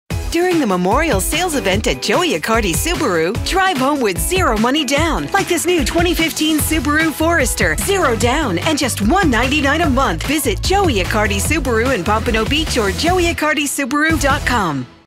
Joey Accardi Subaru - " Memorial Sales Event" (5/2015)
- " Memorial Sales Event " :TV Produced by Herman Advertising. All offers are going for (5 /2015)